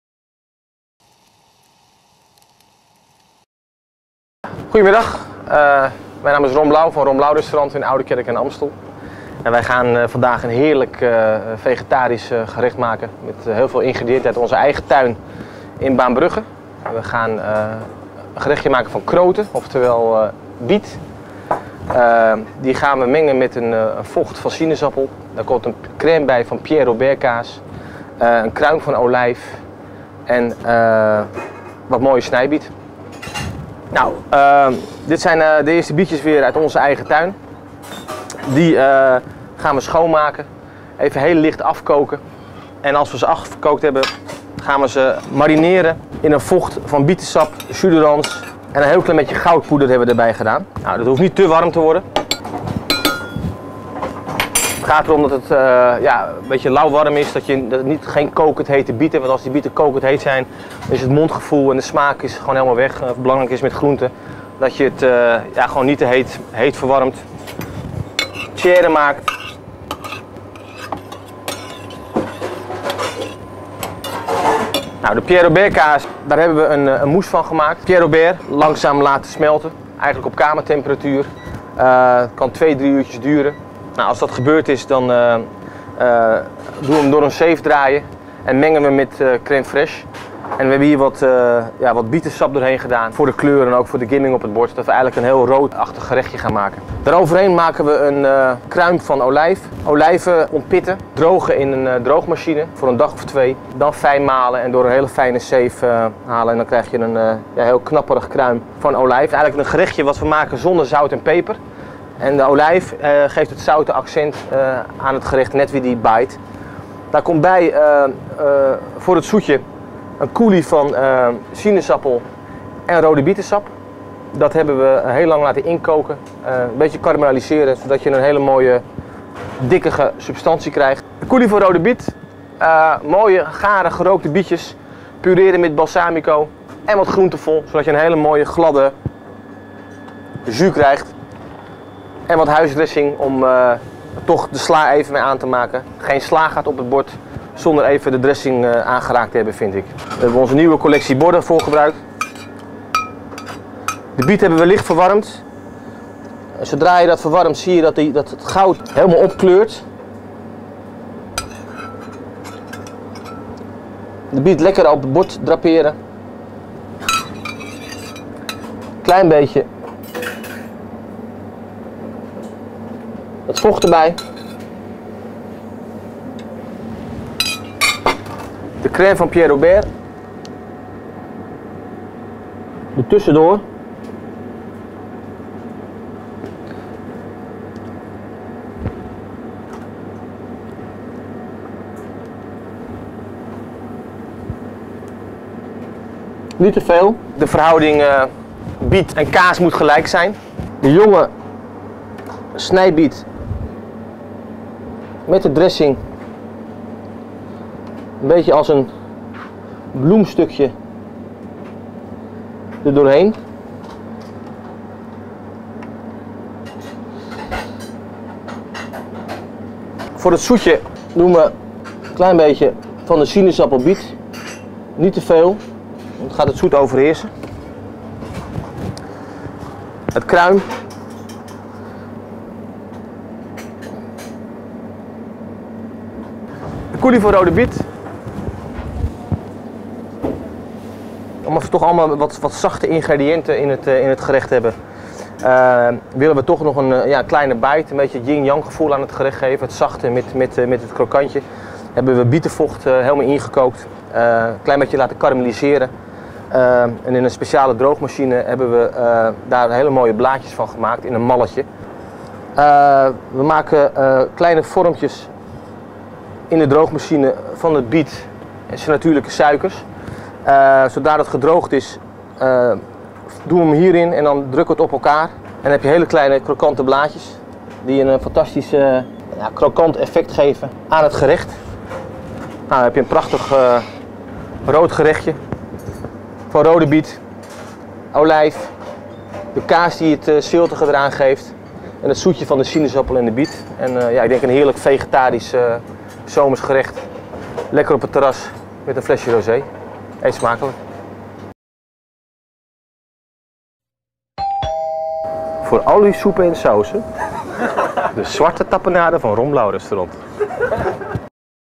Ron Blaauw: biet, goud en Pierre Robert
Twee sterren-kok Ron Blaauw maakt een vegetarisch gerecht van biet met goud en Pierre Robert kaas